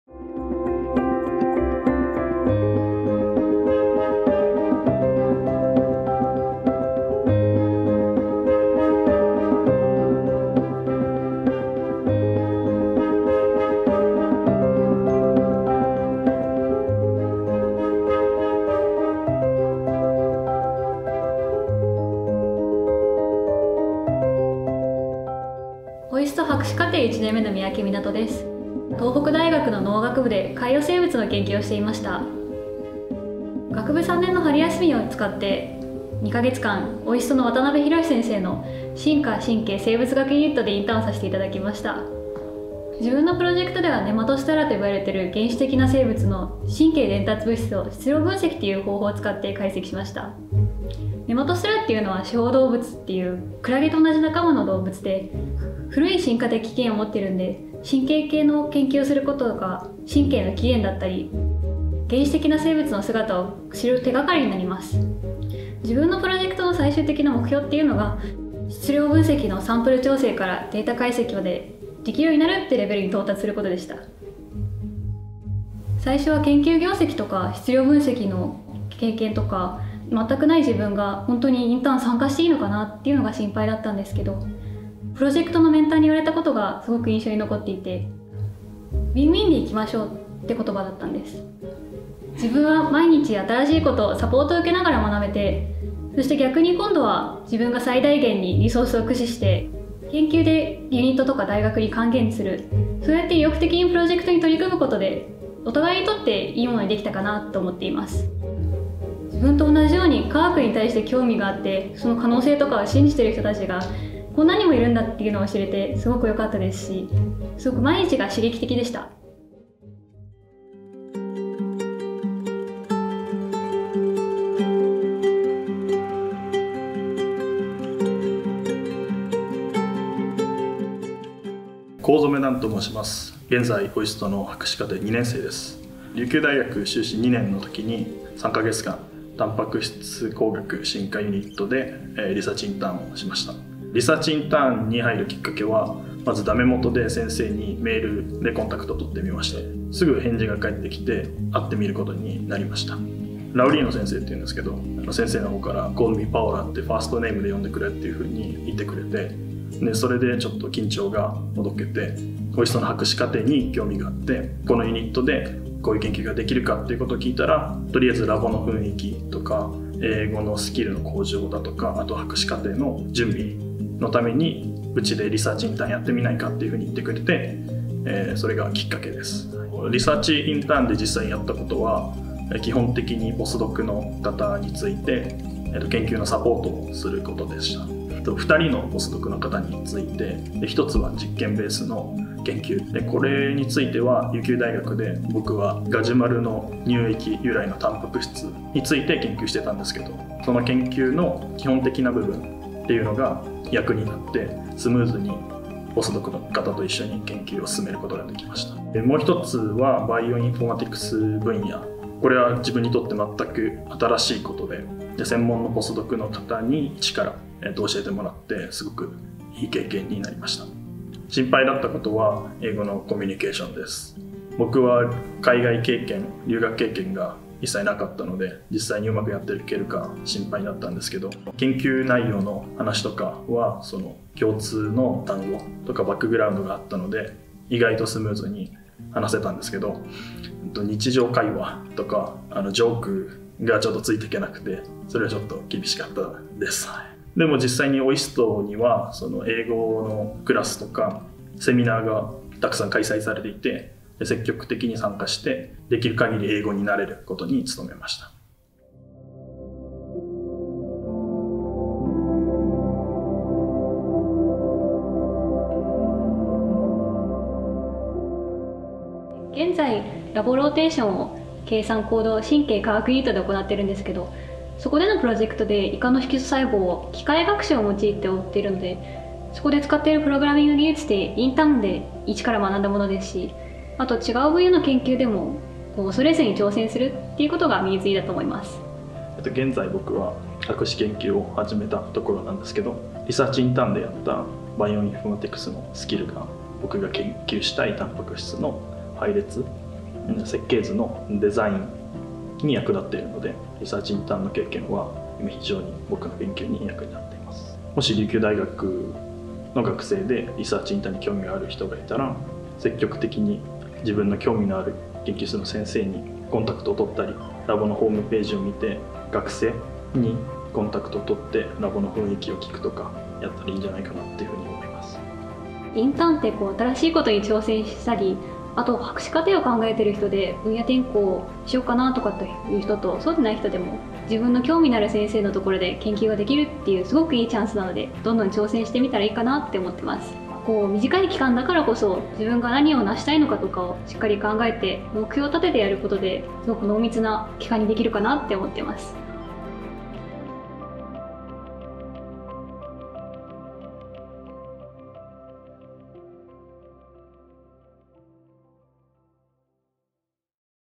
PhD in the Making: Research Internship Program
What is it like to be a Research Intern at OIST? Please meet Minato Miyake and Dan Kozome who joined the Evolutionary Neurobiology Unit and the Protein Engineering and Evolution Unit respectively while they were undergraduate students. After completing the program, they joined OIST as PhD students. How did they apply the Research Internship Program? What kind of research activities can you do as a research intern student? What were their struggles and how did they overcome? Let’s find out!